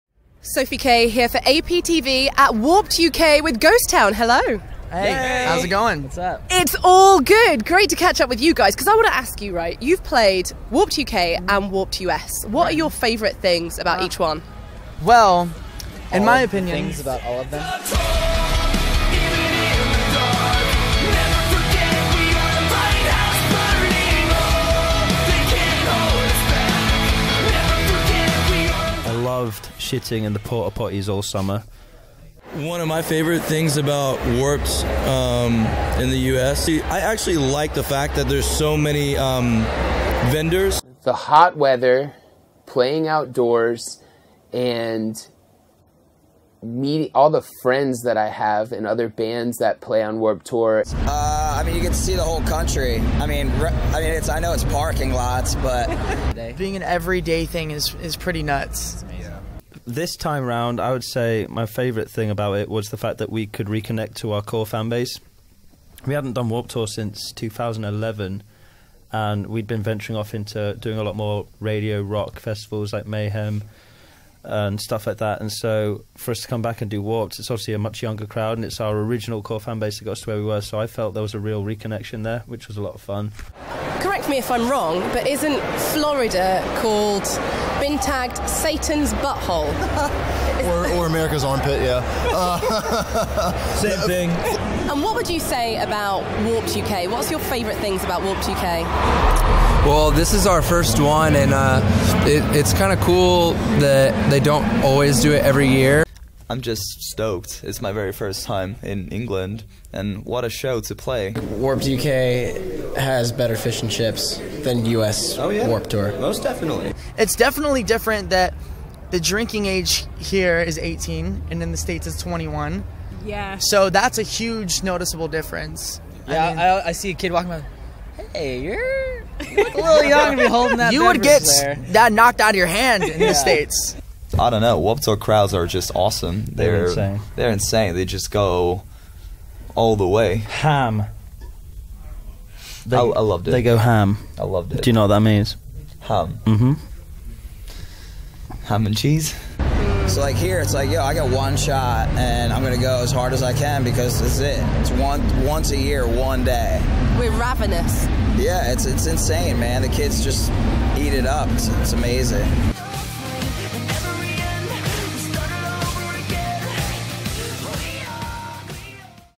WARPED vs WARPED: Asking Alexandria, Attila, August Burns Red, more
How does Warped Tour U.S. stack up against Warped Tour U.K.? Our host Sophie K set out to get Warped Tour U.S. veterans such as Black Veil Brides, Asking Alexandria, Never Shout Never and Attila to compare their international Warped experiences!

The general take-away from Warped U.S. is that the hot weather and traveling the country are standout factors, while Warped U.K. has better fish ‘n chips and amped up crowds for the one-day set. —Jake Hanson

Video/Editing/Interviews: Sophie K
Segment Producers: Cassie Whitt and Ryan J. Downey